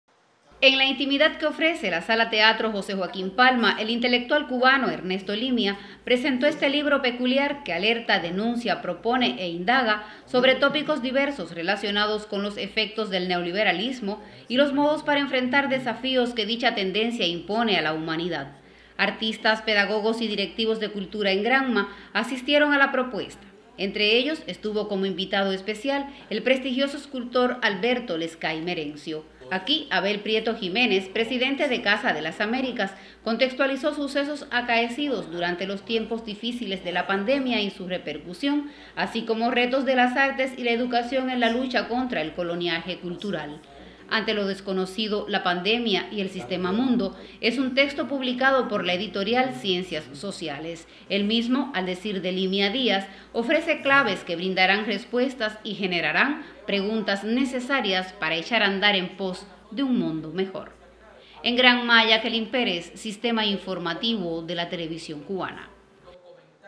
Presentan por primera vez en Cuba el libro "Ante lo desconocido… La Pandemia y el sistema-mundo"
En la ciudad de Bayamo se presentó por vez primera en Cuba el texto "Ante lo desconocido… La Pandemia y el sistema-mundo." El libro, escrito por los intelectuales Ignacio Ramonet, Atilio Borón y Abel Prieto, compila ensayos que enuncian los efectos del neoliberalismo en la sociedad actual.